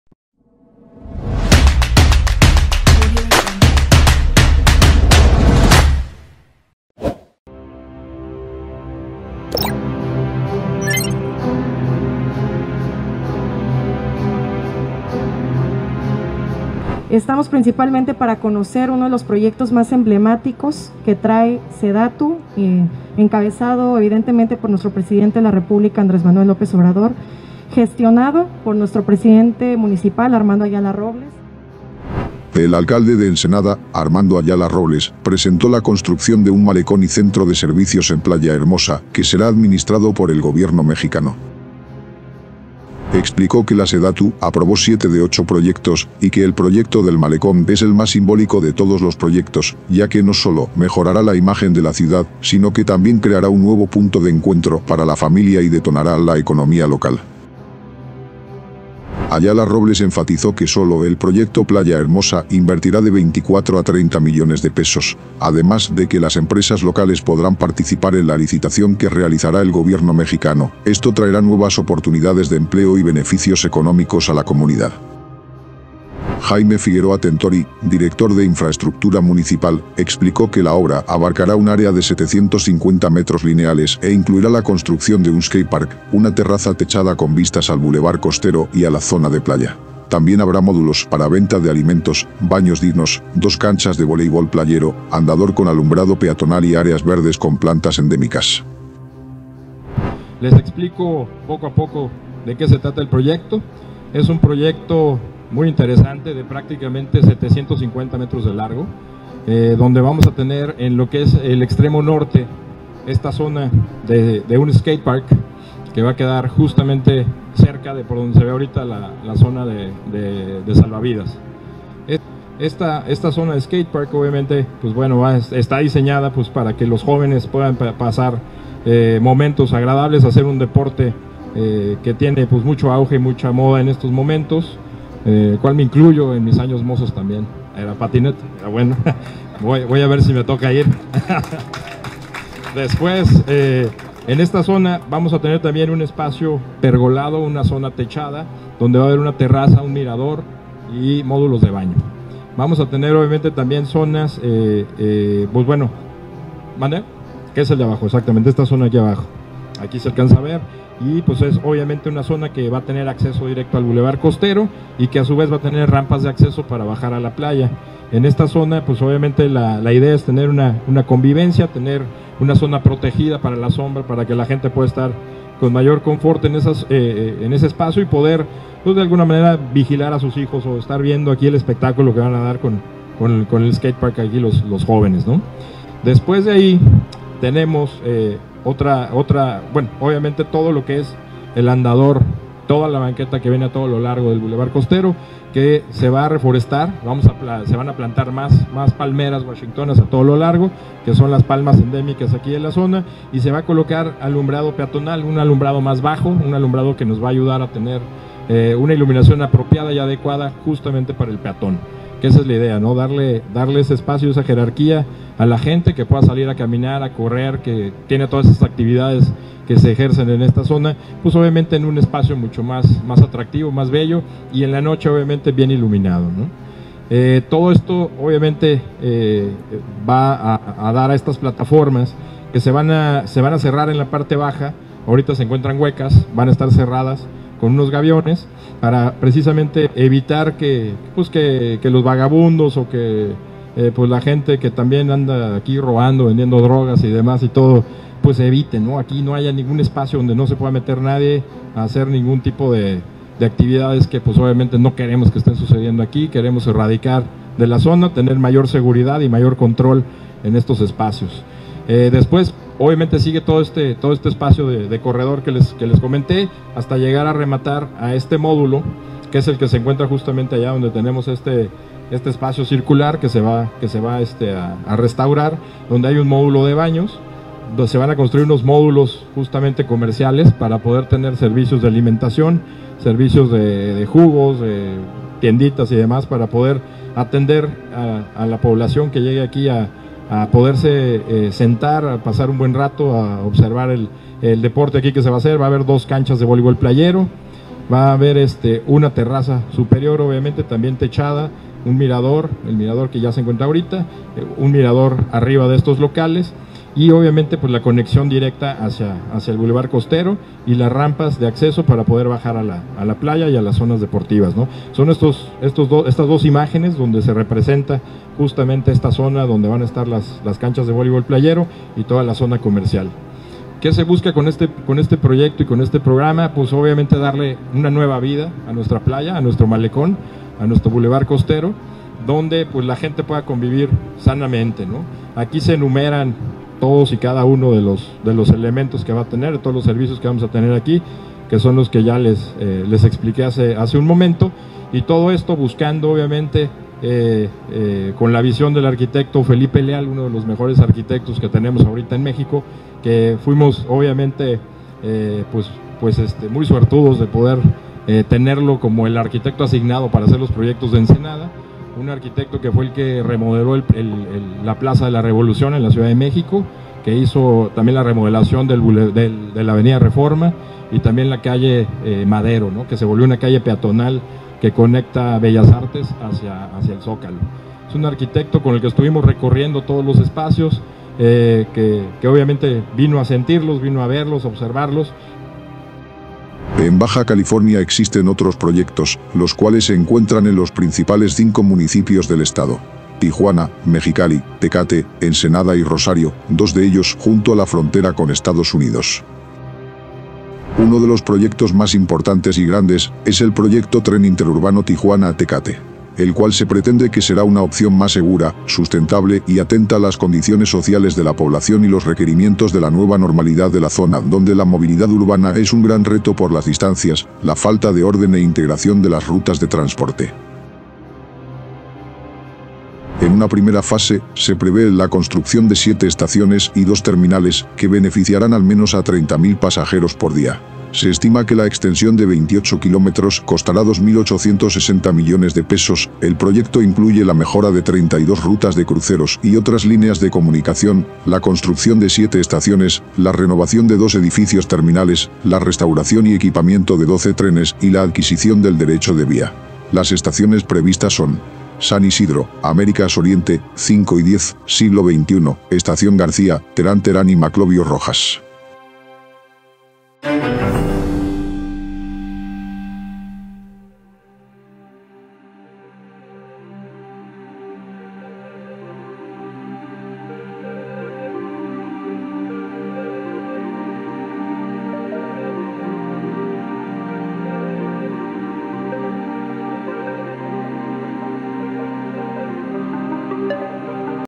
ANUNCIAN LA CONSTRUCCIÓN DE 7 PROYECTOS EN BAJA CALIFORNIA | ENTRE ELLOS UN GRAN MALECÓN
La Sedatu aprobó siete de ocho proyectos, el proyecto del malecón es el más simbólico de todos los proyectos, ya que no solo mejorará la imagen de la ciudad, sino que también creará un nuevo punto de encuentro para la familia y detonará la economía local.

CONOCIENDO MÉXICO (CM): PROYECTOS, INVERSIONES, CONSTRUCCIONES Y NOTICIAS DE MÉXICO.